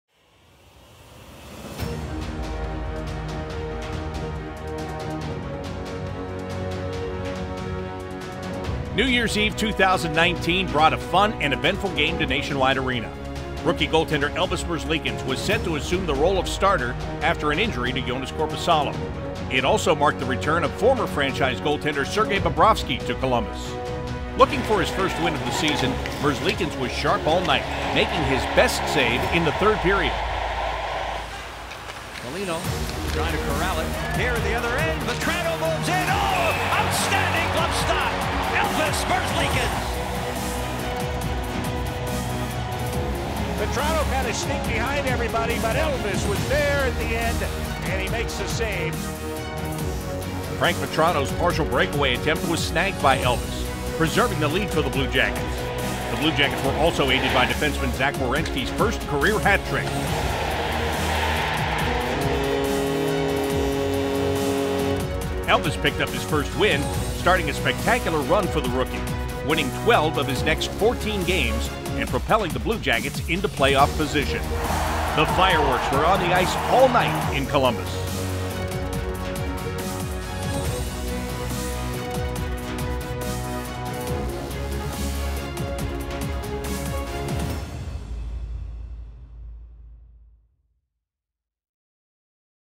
Top Twenty Saves: Elvis' First Win
We're looking back at the best saves in Blue Jackets history, presented by OhioHealth. Today, we marvel at Elvis' first win!